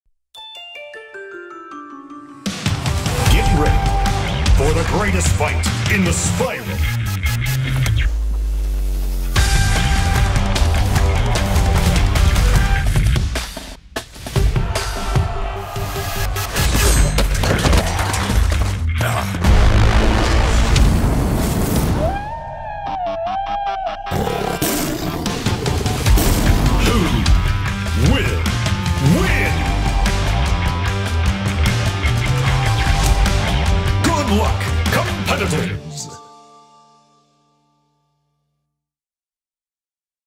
Community Spiral Cup 2023!
The largest Community event of the year is almost here! Have your community leader fill out this form so you can get signed up to participate in 4 exciting challenges. High-performing teams will win 1-month memberships, and the overall winning team will have a mount-variant added to the game based on their design!